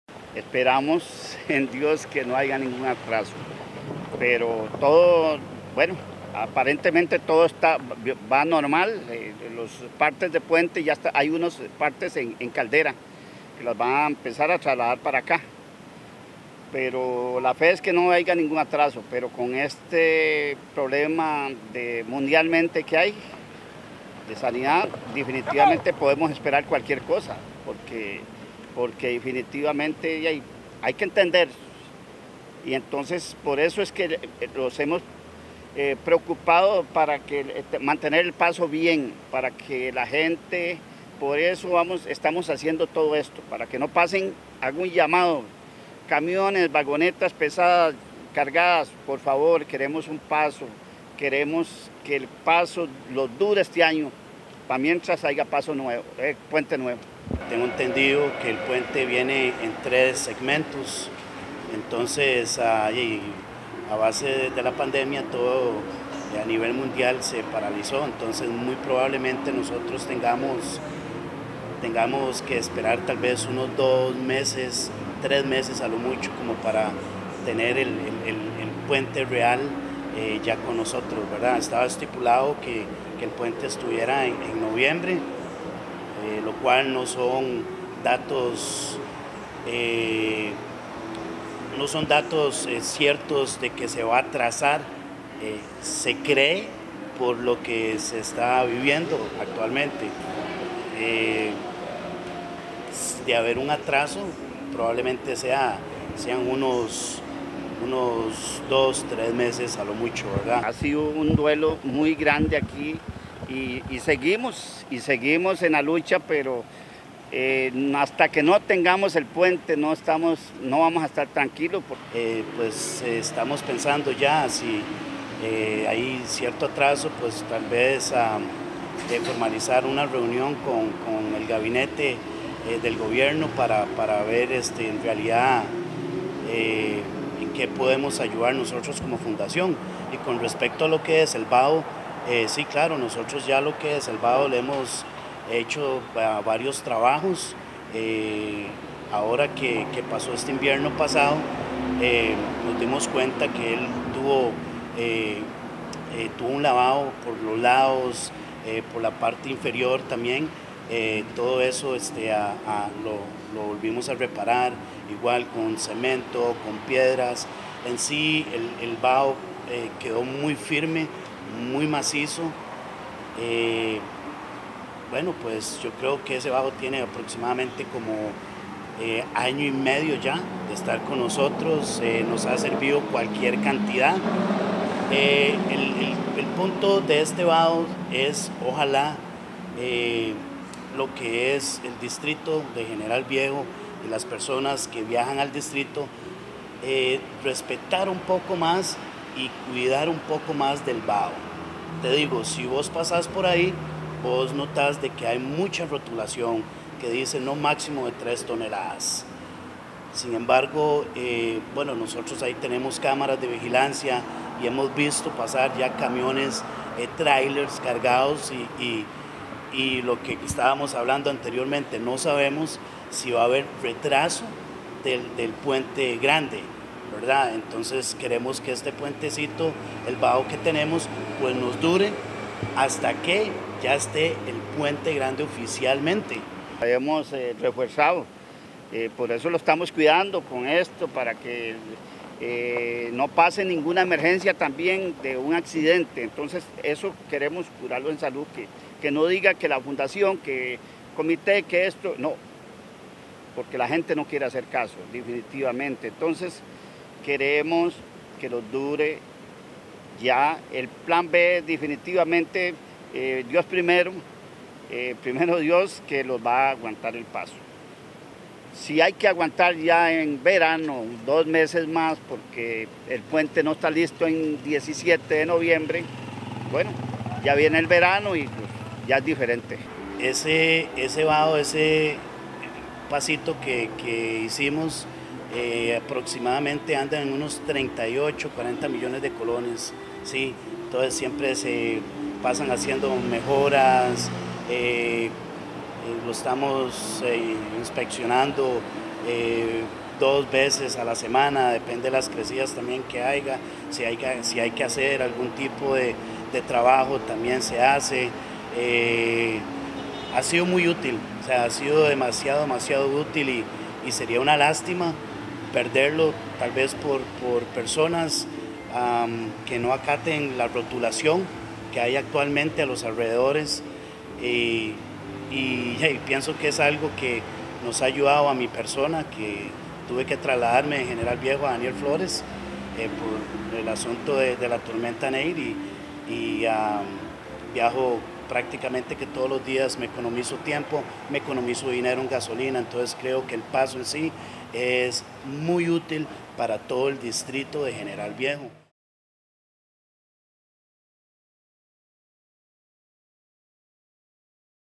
¿Qué planean los vecinos de General Viejo si entrega del puente se atrasa?
Miembros de la Fundación Puentes Solidarios explicaron por qué pusieron agujas en el paso hacia General Viejo y qué planean si la entrega del puente se atrasa.